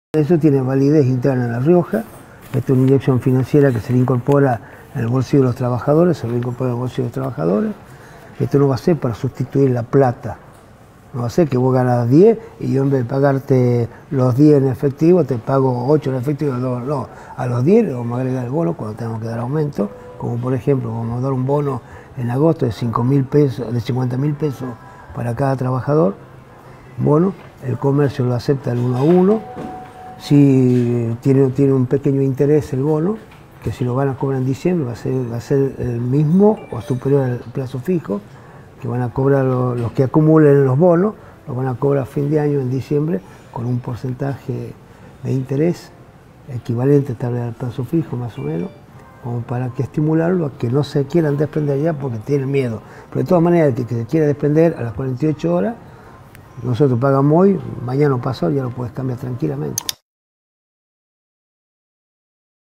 LA RIOJA puso en marcha "EL CHACHO": QUINTELA dio detalles de la CUASIMONEDA
🗣️ El gobernador de La Rioja, Ricardo Quintela, dialogó con Ámbito y dio detalles de la cuasimoneda "El Chacho".

🎯 "Esto es una inyección financiera que se le incorpora al bolsillo de los trabajadores. No va a ser para sustituir a la plata", expresó.

Por @ariel.basile | @gabrielmorini83

🗞 Leé